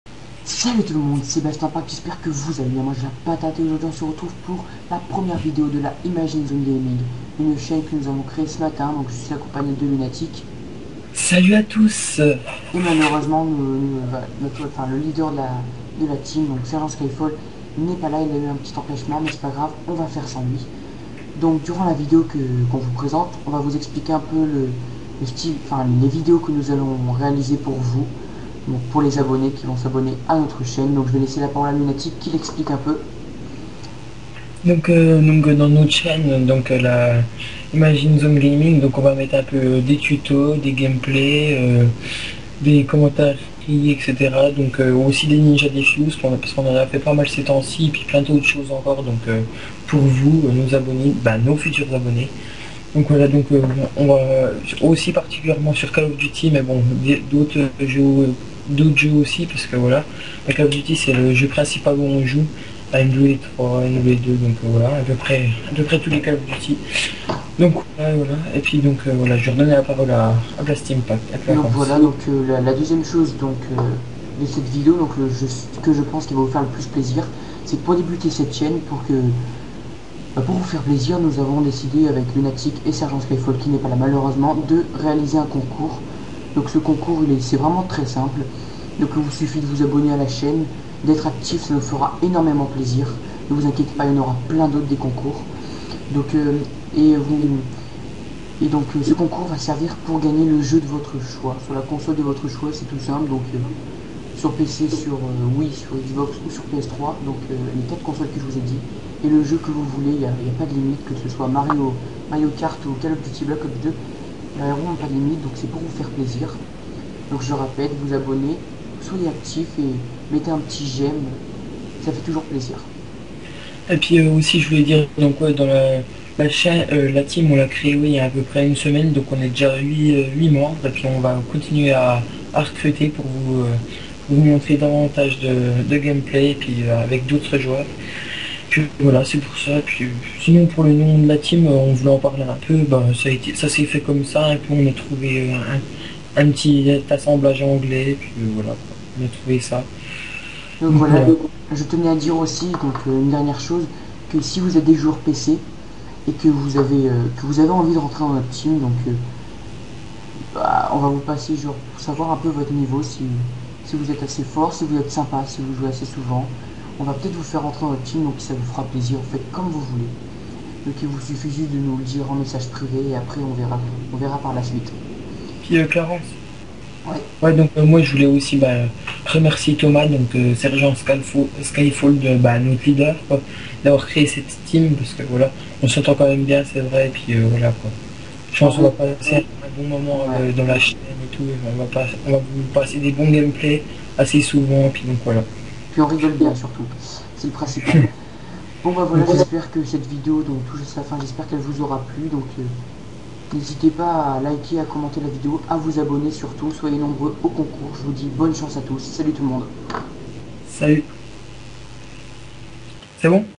/!\ Présentation de la chaine /!\
/!\ LIRE DESCRIPTION /!\

Aujourd'hui la première vidéo de notre chaine IzG' ImagineZoneGaming
Présentation + concours !

Pour participer :

1/ Commenter la vidéo avec le jeux de votre choix et la console
3/ Soyez actif sur la chaine

Merci a tous ! =) Sinon a toute pour notre prochain concours a nos 100 abonnés.